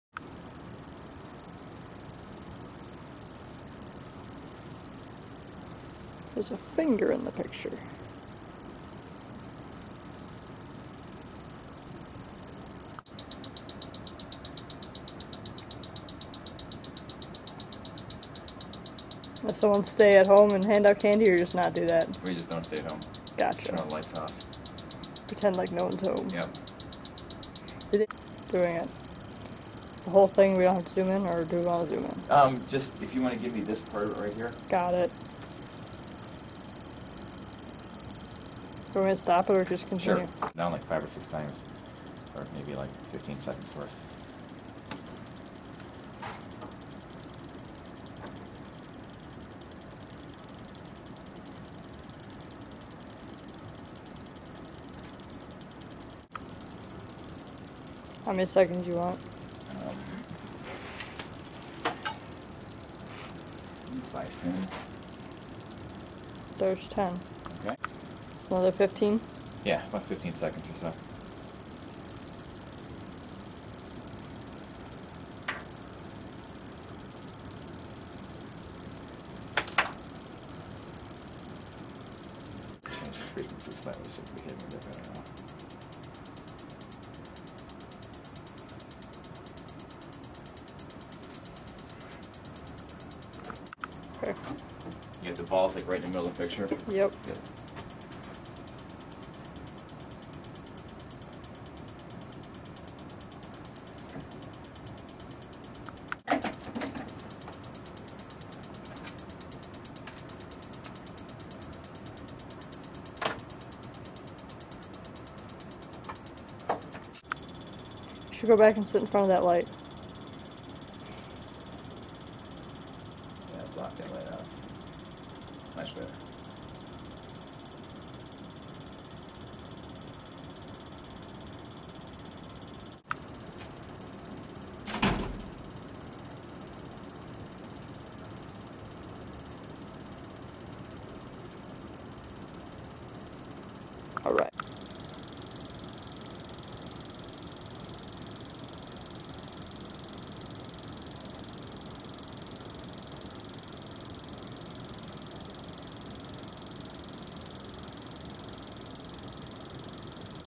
Modes of Vibration for 1-dof, 2-dof, 3-dof systems
Demonstration of modes of vibration for 1-dof, 2-dof, and 3-dof mass-spring systems using whiffle balls and rubberbands. A previously posted (but more recently recorded) version of this demonstration used a high speed camera to record the motion.
In this raw video clip (recorded around 2011) the actual speed of the oscillation is explored with a strobe light, in order to contrast the actual speeds (which are hard to see in real-time) with the strobed sampling which highlights the relative phase of the balls while oscillating in the various modes.